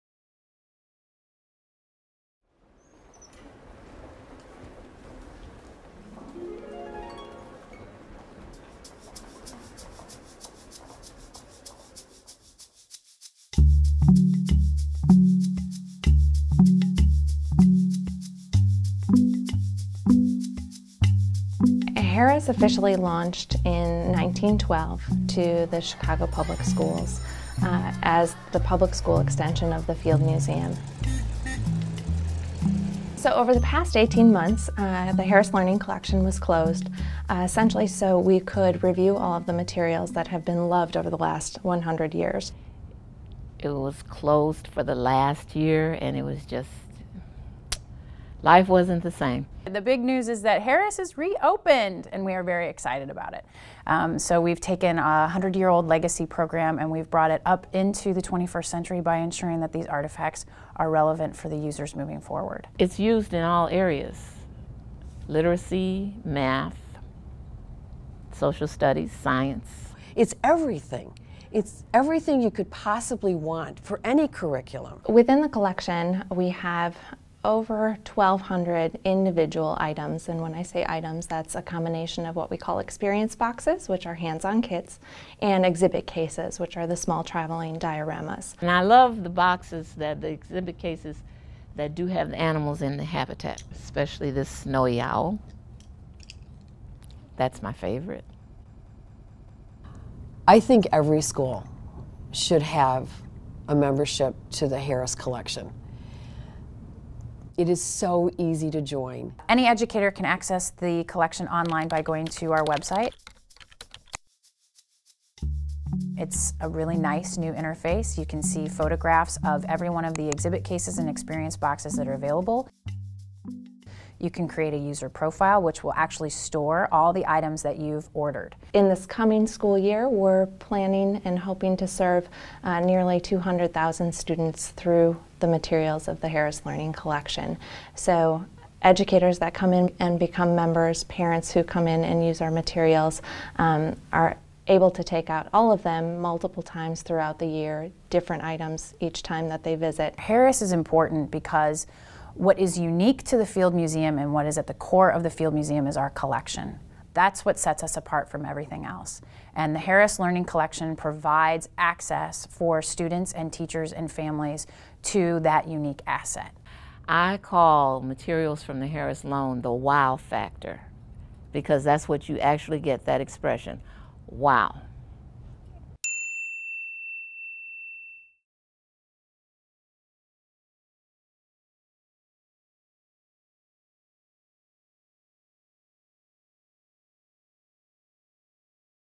The Harris Learning Collection
In October of 2012, the Field Museum's Harris Learning Collection re-opened its doors to the public and Chicago area educators. Learn about Harris' amazing historical resources and how they can supplement your child's education, at home or in the classroom.